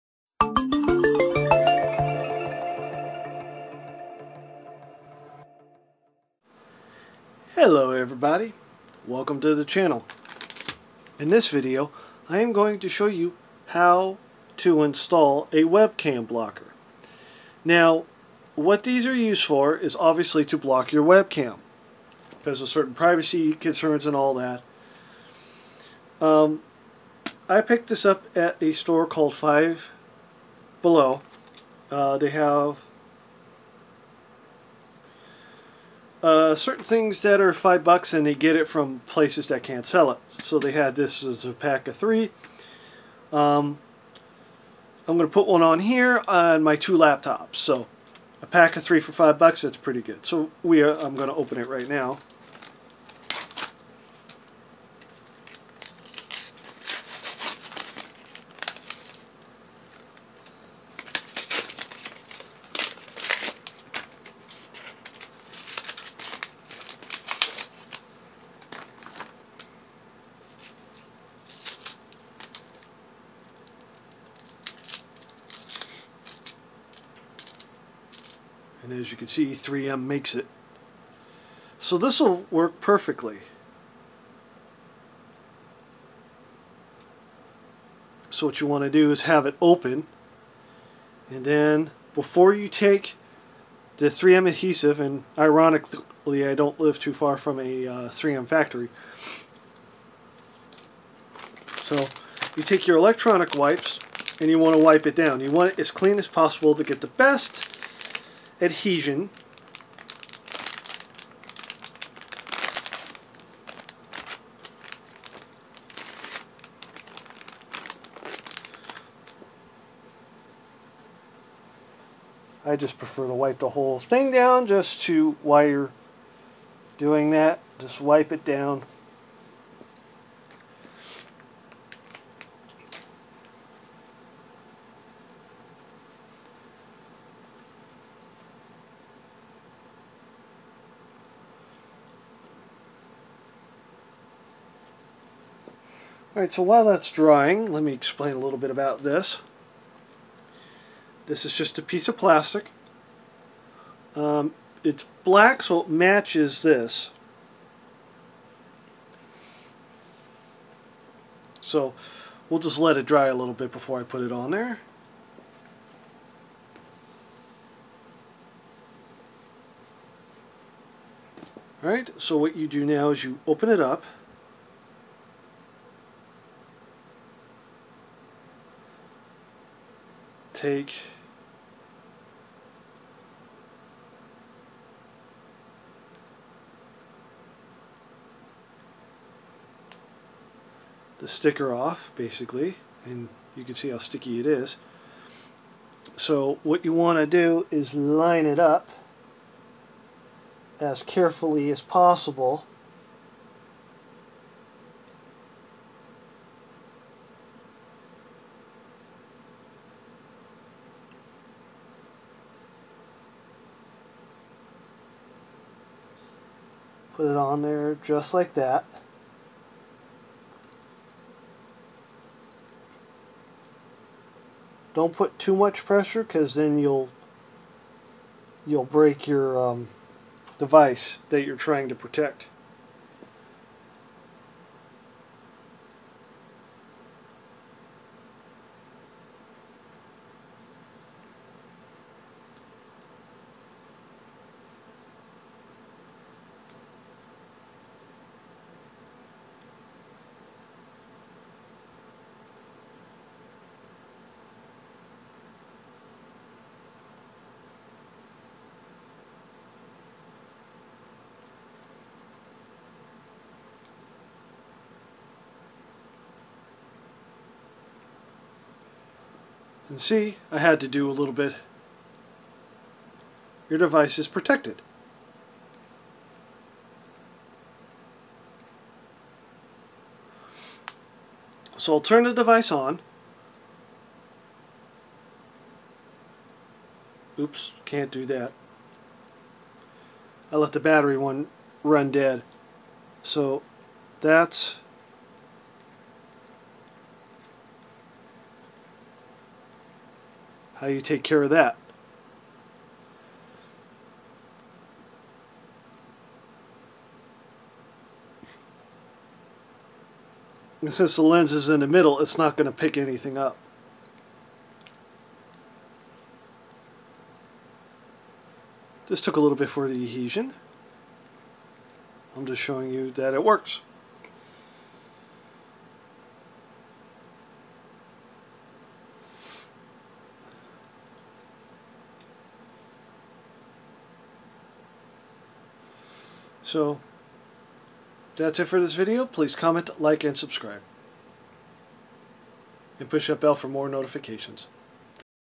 camera lens cover for your phone or tablet
this particular camera cover is slim in desighn and peel n stick